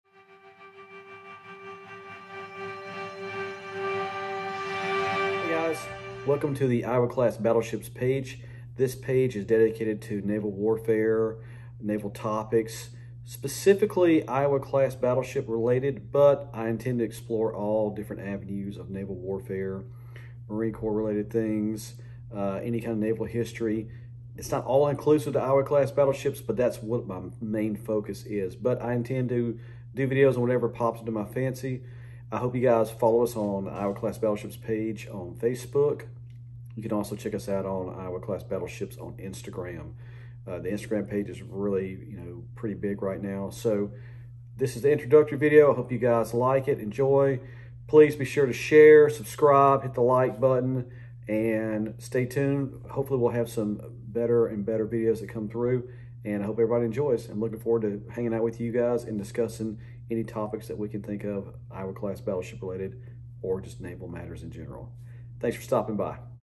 Channel Intro
Welcome aboard to the Iowa Class Battleships page!  We will be covering all things Iowa class related to include Naval warfare, various ships, and any aspects of Naval and Marine matters.  I look forward to hearing from you all.